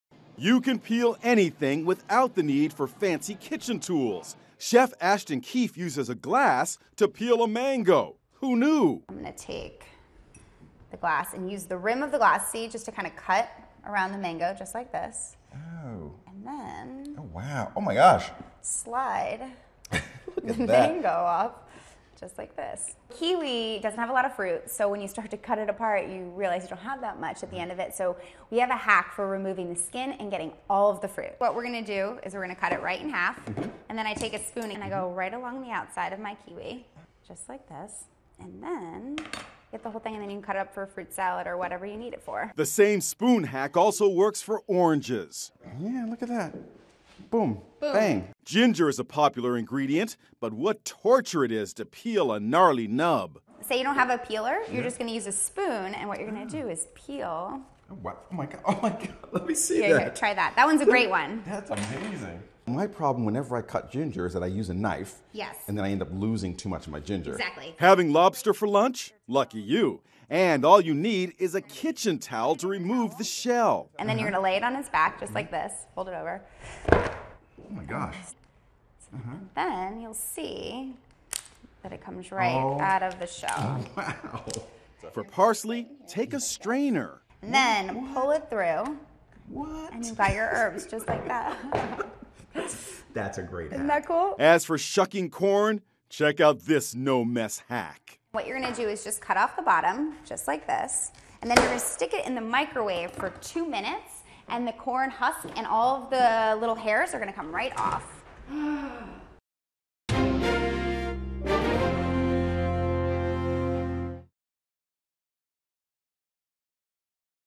How to Peel a Mango Using a Glass
Forget the fancy cutlery. Home cooks can peel anything with tools they already have in their kitchens. Chef Ashton Keefe uses a drinking glass to peel a mango. And he uses a spoon to scoop out kiwi from its skin, or an orange from its rind. Did you know that a spoon is also the secret weapon to peeling ginger? And how can amateur and professional chefs alike get to cooking that lobster, garlic or corn cob faster? The tricks are right in front of you.